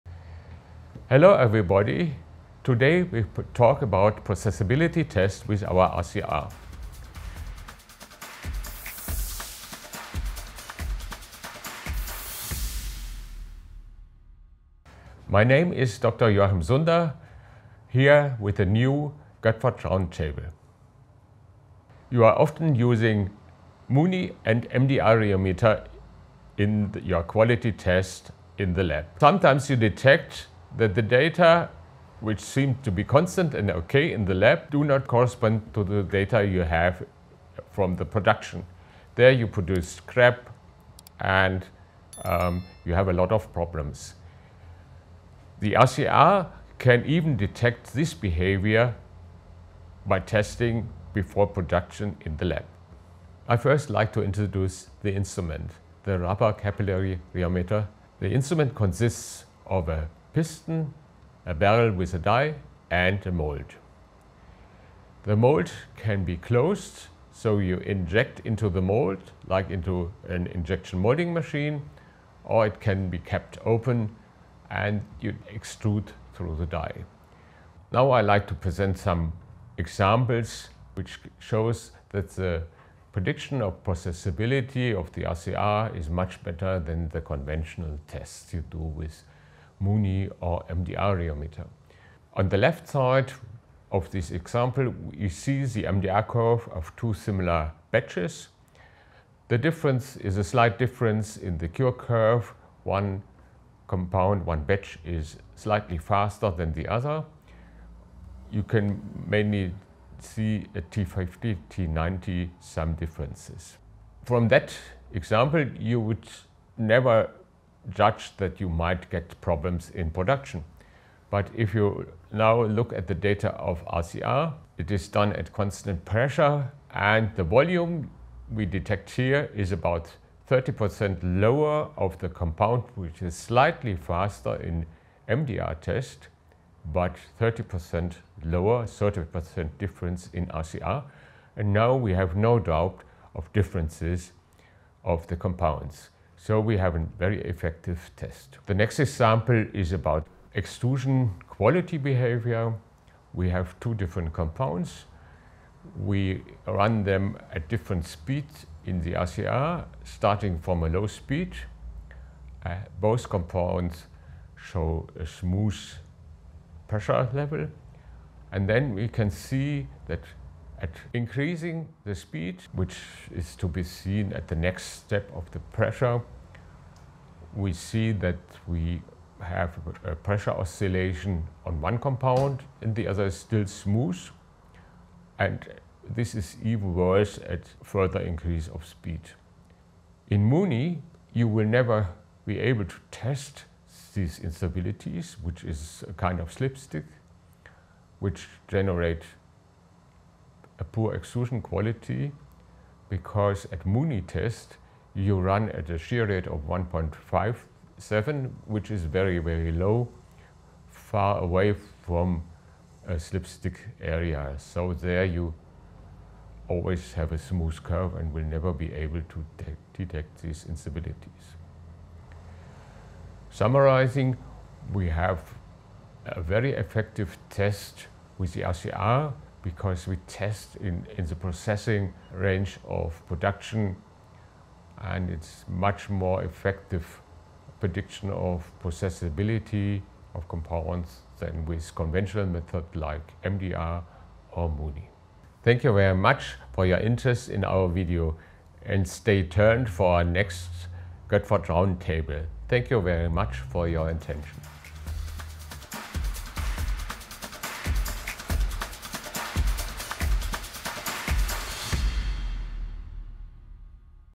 Solve production problems in advance | Rubber-Capillary-Rheometer RCR
A capillary rheometer is designed to determine the flow and vulcanization behavior of rubber compounds in a short test under processing conditions. The measurement of thermosets is also optionally possible.

The RCR is optimized for the shortest possible measuring time and easy operation to provide very fast results especially in QC applications. A typical measurement takes about 2-5 minutes.

► Who is GÖTTFERT?